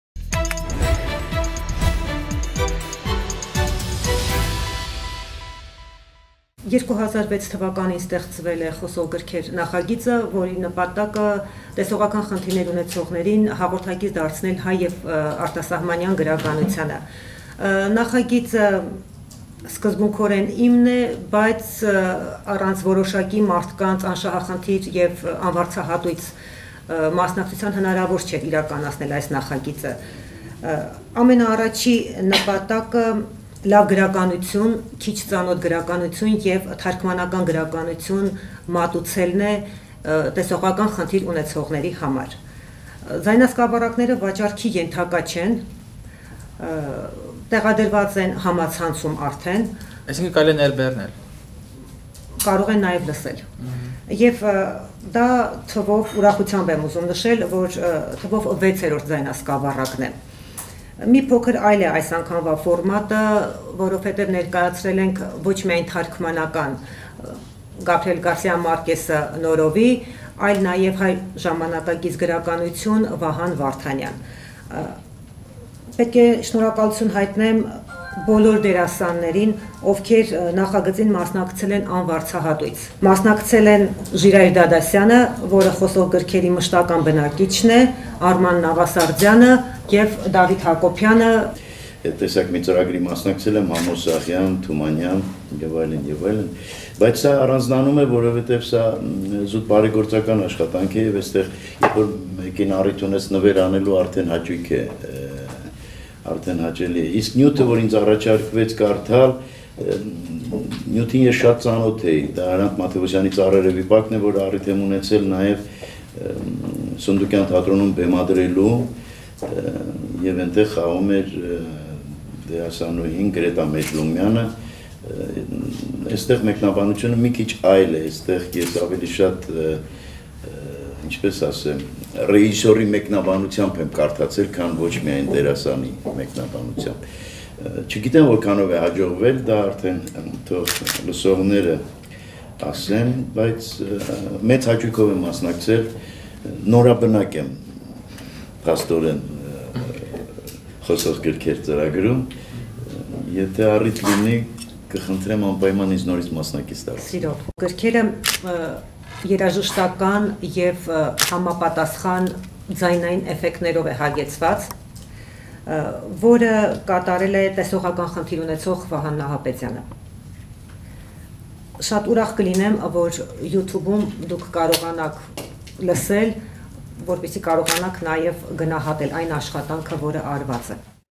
Հայերեն խոսող գրքեր
Տեսողական խնդիրներ ունեցողների համար լույս է տեսել «Խոսող գրքեր»-ի 6-րդ ձայնասկավառակը | HotTV լուրեր